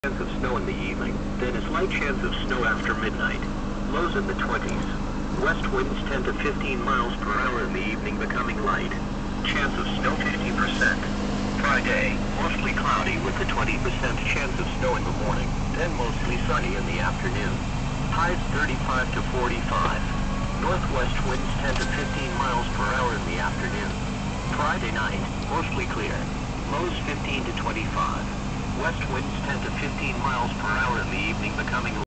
Think it will snow?
Listen to the National Weather Service in Grand Junction on the radio. Gary and I were coming back from Austin in my little green car. We stopped at the convenience store for something to drink and it got worse from there.  South on Highway 50 got so bad I had to fall in line behind a tractor trailer for about three miles so I could follow the tracks in the road.